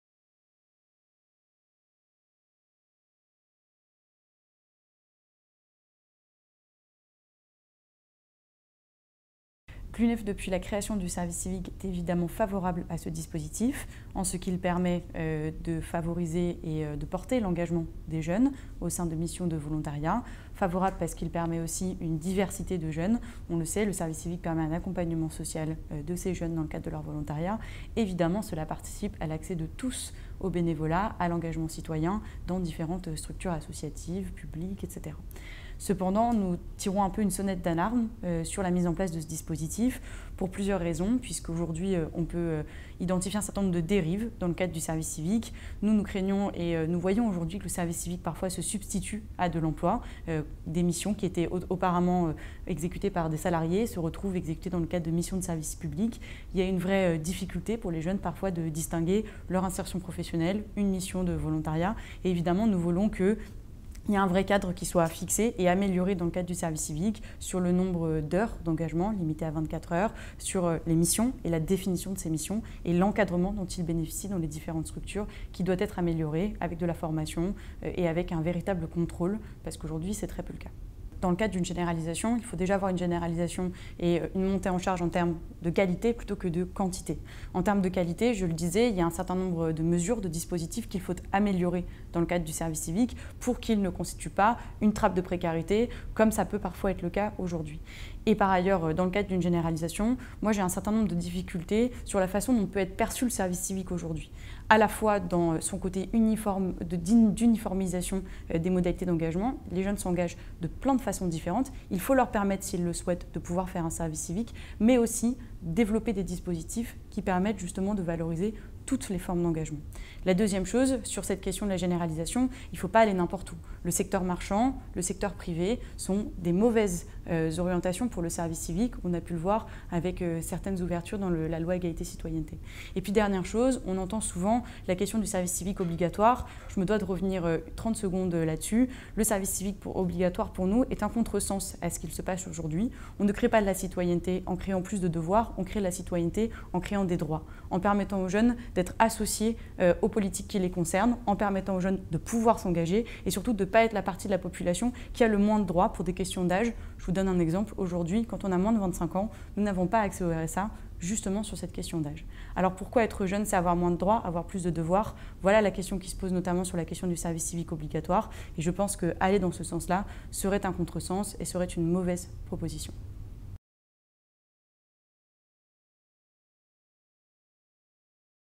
Questions à Marthe CORPET (UNEF) - Service Civique - cese
Questions à Mme Marthe CORPET, Vice-Présidente de l’UNEF, entendue au CESE à l'occasion d'une Table ronde avec les représentant.e.s des mouvements de jeunesse dans le cadre de la saisine : "Service civique : Quel bilan ? Quelles perspectives ?" conduite par la Délégation à la prospective et à l'évaluation des politiques publiques.